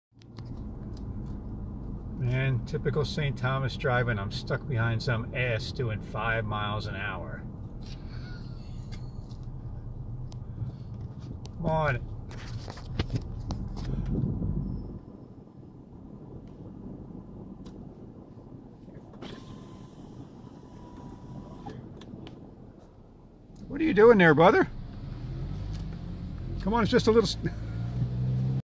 Car Stuck Behind a Donkey Doing 5 Miles an Hour
Location: Virgin Islands, U.S